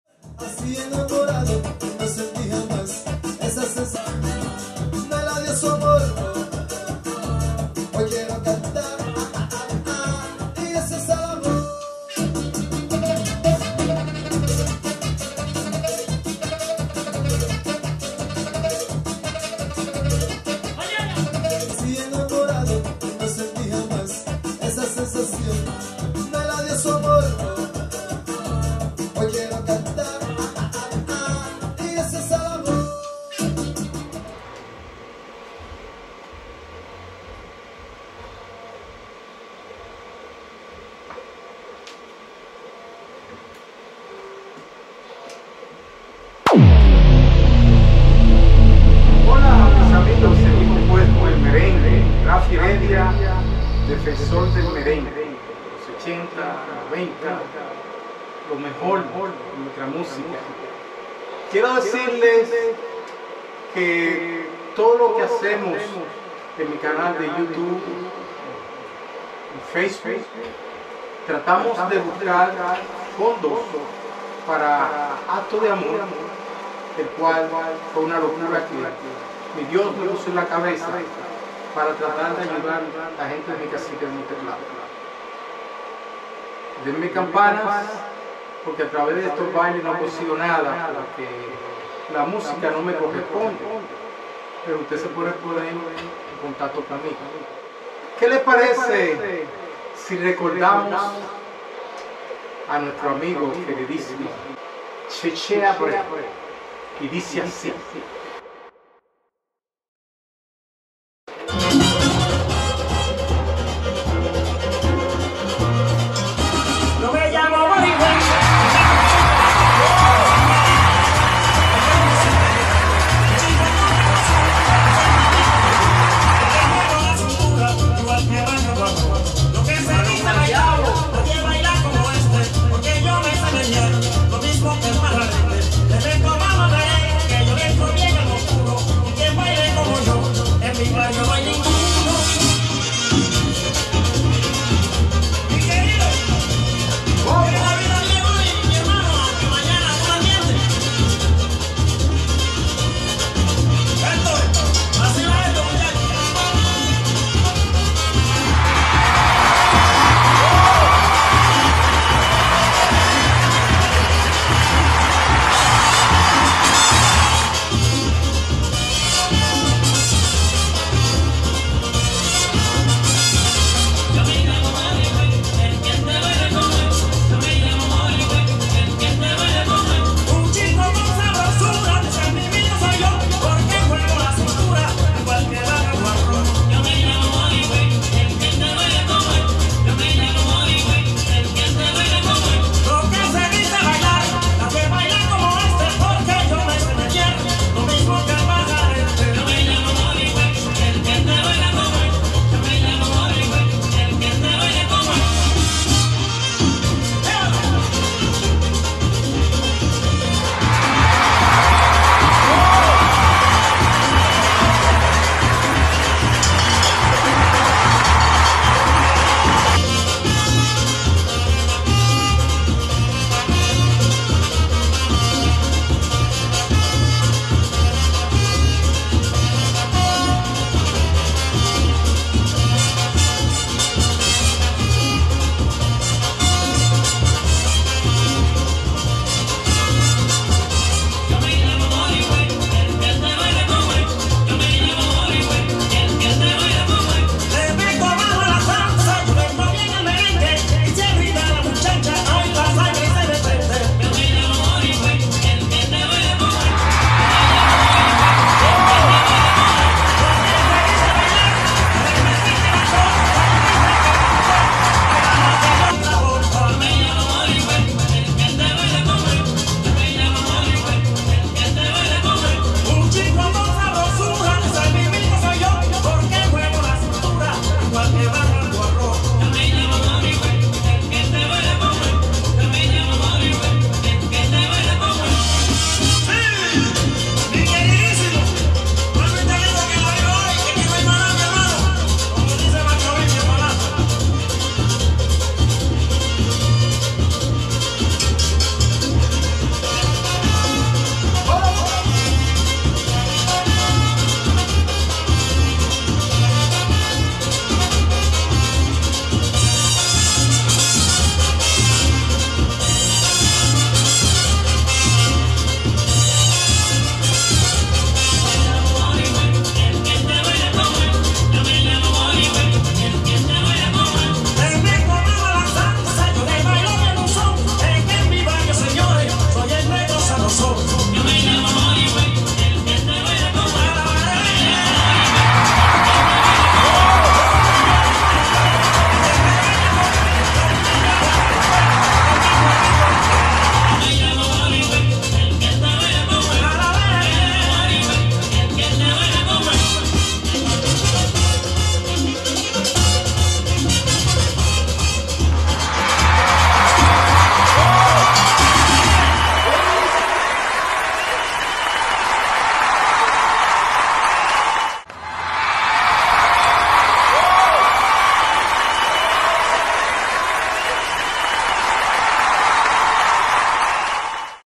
CAPSULA # 337 " RAPHY HEREDIA DEFENSOR DEL MERENGUE # 7 "
RAPHY HEREDIA BAILA MERENGUES CON UN TEMA MUSICAL DE CHECHE ABREU , QUE VIVA EL MERENGUE .